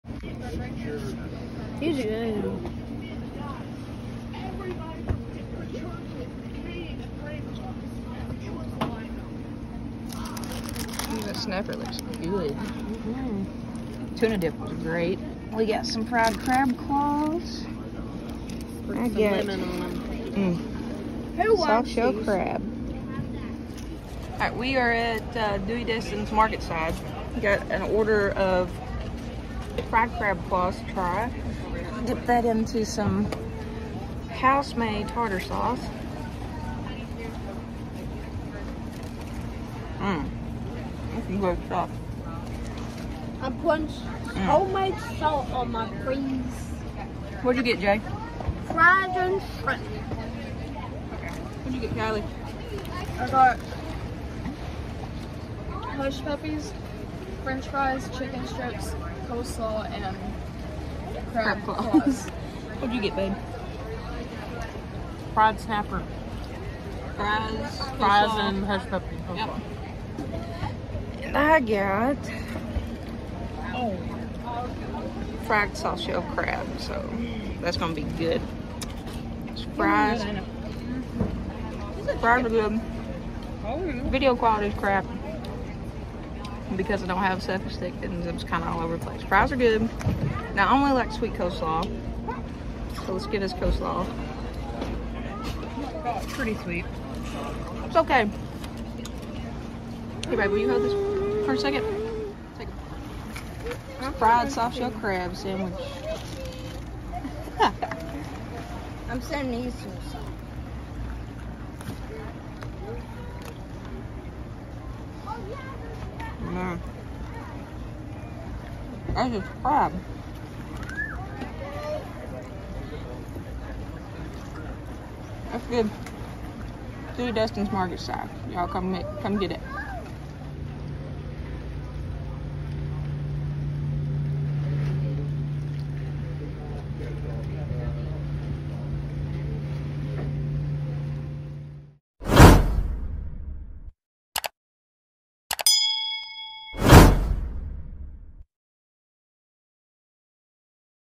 Dewey Destin’s Seafood Was A 10/10!
Dewey Destin’s Marketside was 🔥🔥🔥 Everything else was A 10/10 for sure here! The smoked yellow fin tuna dip was 🔥. My soft shell crab sandwich was amazing! Syd got the fresh catch fried snapper basket which melted in your mouth. Jay’s shrimp was also fresh and really good. Kylee got a chicken strip basket and she said it was great. The fried crab claws and that peanut butterfinger pie was on point! Also, the view we had was 100/100!